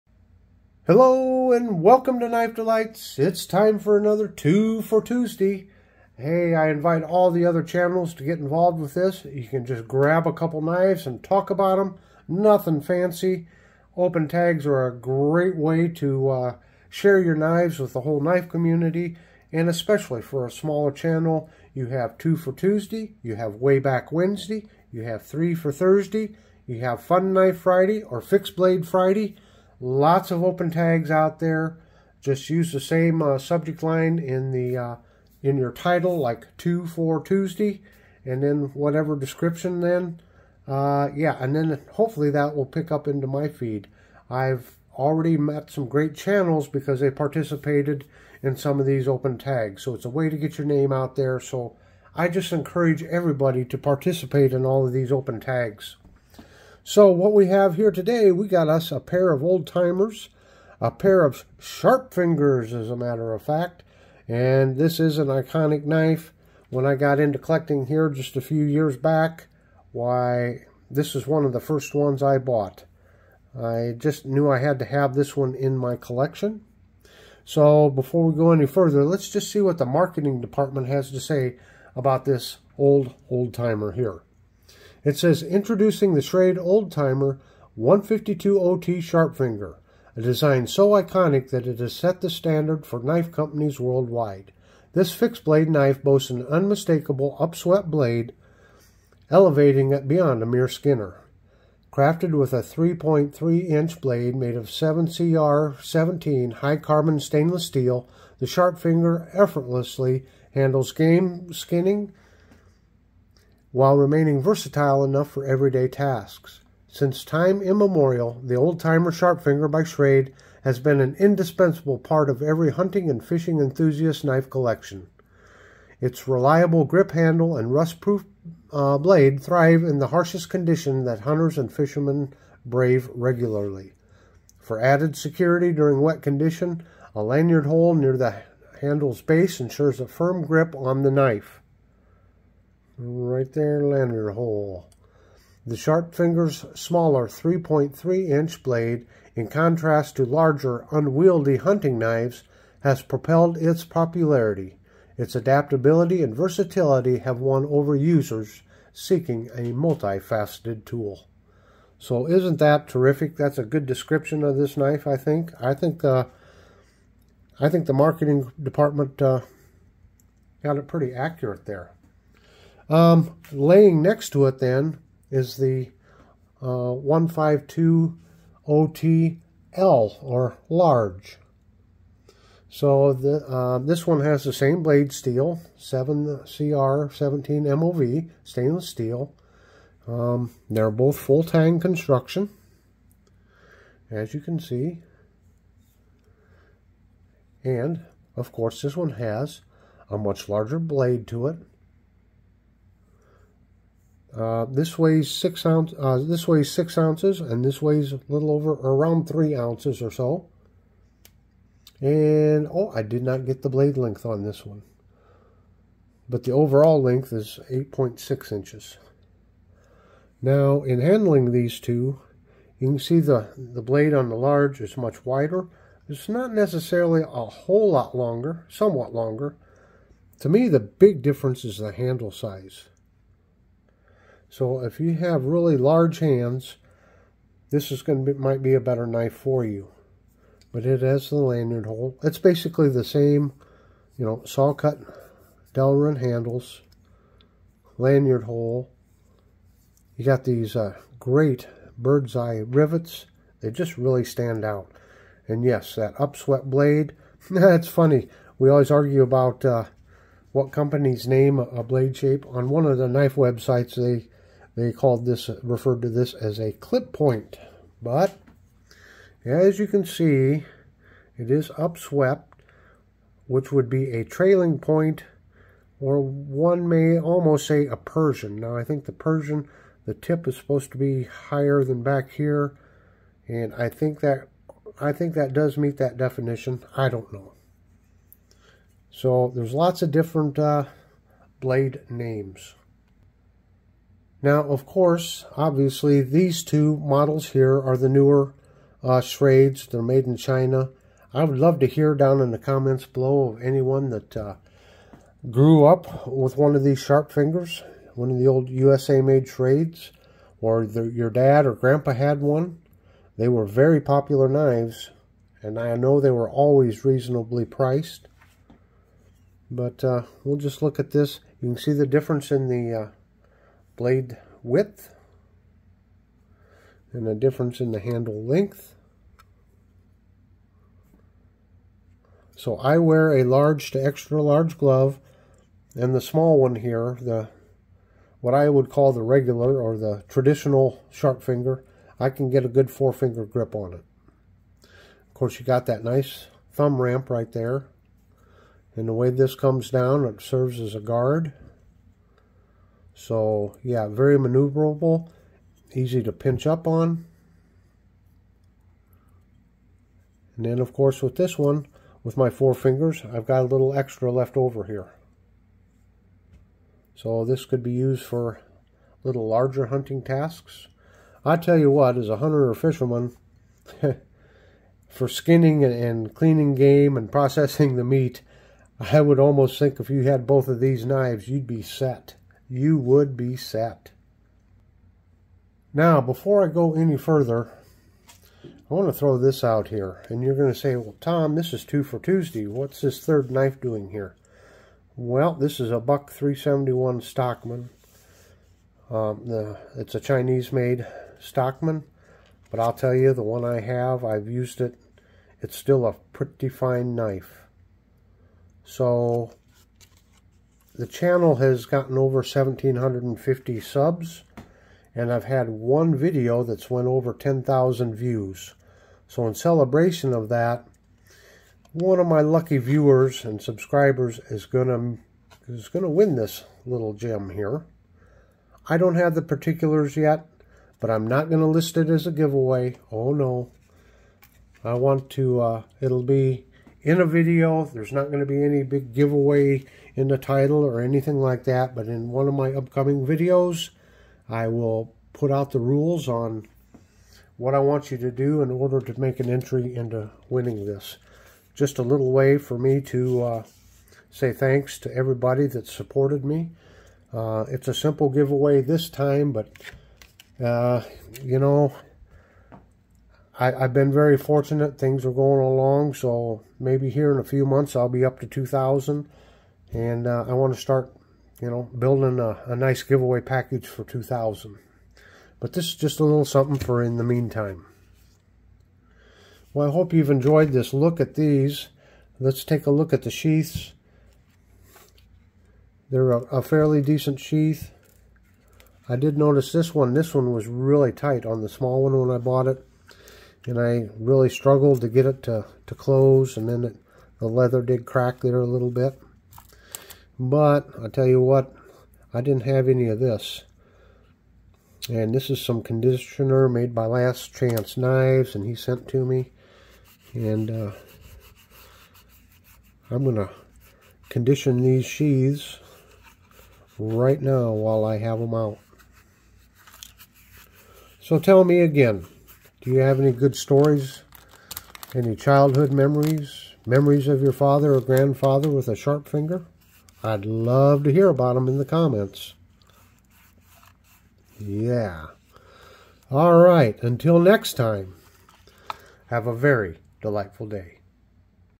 Two for Tuesday: Review of the Old Timer 152OT & 1520TL Sharpfingers
A look at this iconic knife pattern that is sure to bring back great memories for some!  The large (152OTL) was gifted to me by RJ's Knives.  Thanks again, RJ!  Enjoy the video.